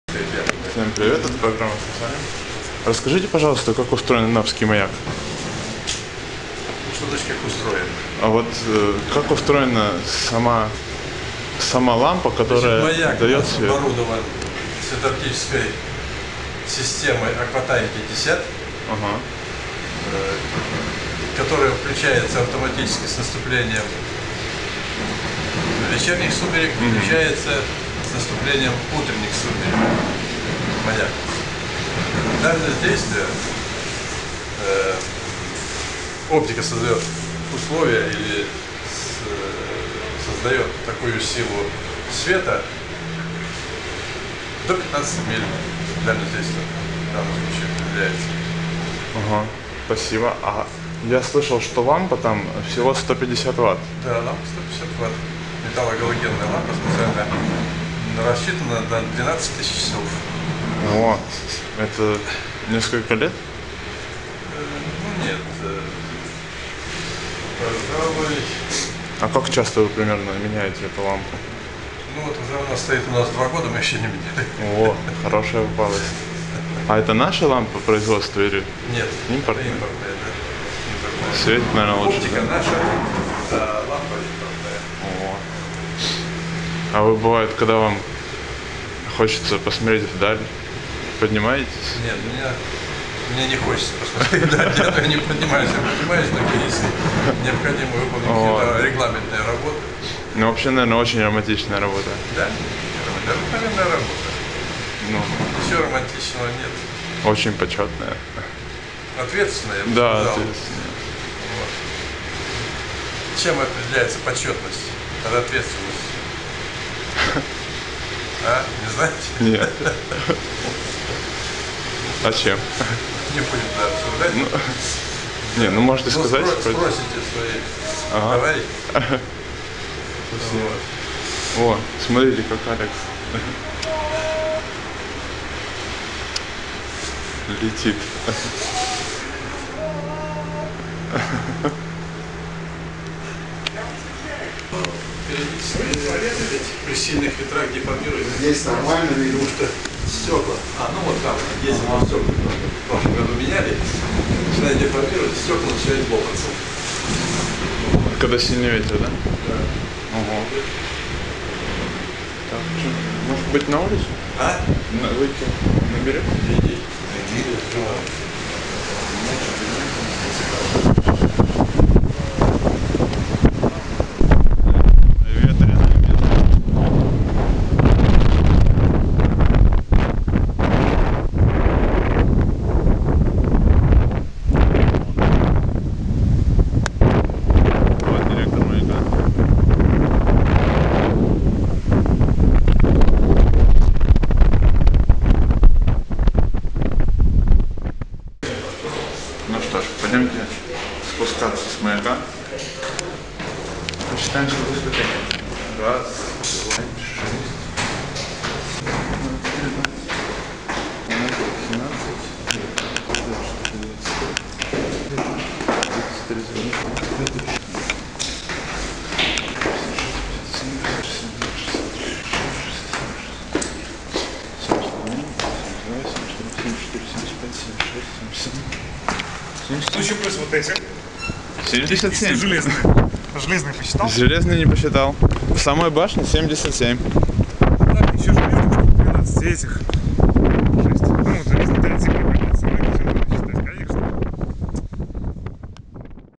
Маяк Анапский | Anapskiy Lighthouse
6-го января 2012 года месте с Алексом Дубасом и съёмочной командой телеканала "Моя планета".
Нас встретил начальник маяка Анатолий Фёдорович Нараевский, приятный мужчина, лет 50. Дал небольшое интервью, потом мы поднялись в башню.

Маяк установлен в 1955 году на обрывистом берегу мыса Анапский. Восьмигранная башня, с тремя черными горизонтальными полосами. Высота центрального огня от уровня моря 45 метров. Свет огня красный, группопроблесковый. Видимость в ясную погоду до 15 морских миль. Маяк анапский оборудован светооптической системой акватайм 50, которая включается автоматически с наступлением вечерних сумерек и выключается с наступлением утренних сумерек. Специальная металло-галогенная лампа на 150 ватт, рассчинанная на 12000 часов. Кроме того маячный комплекс оснащён современной системой радиосвязи и дизельным генератором.
К сожалению маяк не дошёл до наших дней в первоначальном виде, был разрушен немецкими войсками при отступлении в 1943 году. С 14 июля 1898 г. подходящим к анапским берегам кораблям стал светить огонь керосиновых ламп, установленных в деревянной будке на козлах. Это был временный маяк-мигалка, изготовленный по принципу шведских маячных огней. 20 октября 1909г. торжественно был зажжён новый маяк, оборудованный в красивом башенообразном строении (фото
С башни маяка открывается красивый вид на морской пейзаж и подножие Кавказских гор. Рядом находится старое городской кладбище, а по береговой линии тянется длинная набережная.

Радиосвязь с маяками.
Anapskiy (Black Sea) Light, Anapskiy Lighthouse, Анапский, RLE-048( RLHA-Nr), R6A(Reg), ERU-295(ARLHS-Nr), KN84pv(Gridsquare), UA6LP(QSL Route), Admiralty E5618; NGA 18944 Черное море  44*53'N 37*18'E

The Anapa's lighthouse in 1955 on cape Anapa has been established. The form of a tower of the lighthouse - octahedral, with three black horizontal strips. Height of a signal light of 45 metres above sea level, colour red. Visibility of this beacon at a fair weather about 15 n miles. Except a signal light, the Anapa's lighthouse is equipped modern radio engineering by means. Unfortunately, that beacon has not reached us in the original form as has been destroyed by fascist invaders at deviation. And the Anapa's lighthouse has been based on July, 14th, 1898. The cut of high coast had an earthen hill (the rests of an ancient barrow), on it the first alarm beacon has been put, and in operation the new beacon a flasher on October, 20th, 1909 has been placed. The beacon always is an integral part of a sea landscape. And the Anapa's lighthouse not an exception, after all it is at bottom of mountains. From high coast we can observe a picturesque landscape. To the southeast from a city in 5 km spurs of the Big Caucasus tower. They begin with Bald mountain and pass in a ridge Itself.